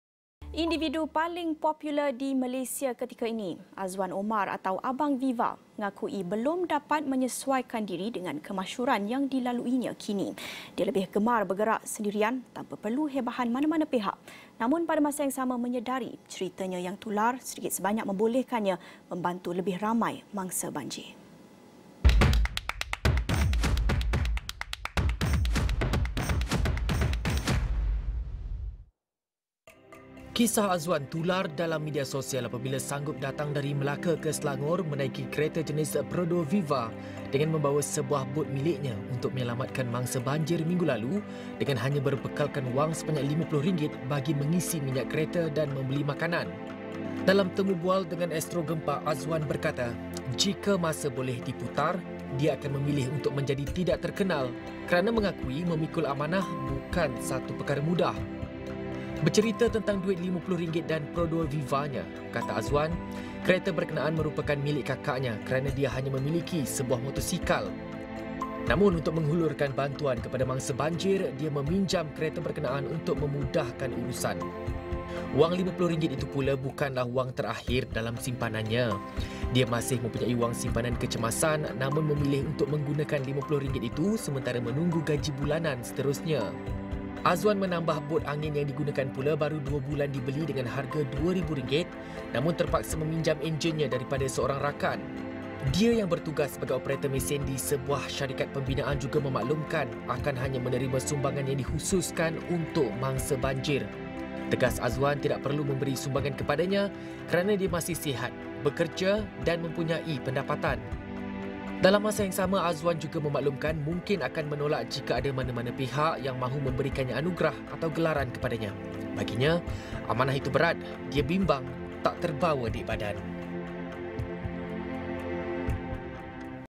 AWANIxGEMPAK | Di mana ada banjir, di situ ada Azwan
Setulus hati Azwan Omar..
Individu paling popular di Malaysia ketika ini, Azwan Omar atau abang Viva, mengakui belum dapat menyesuaikan diri dengan kemasyuran yang dilaluinya kini.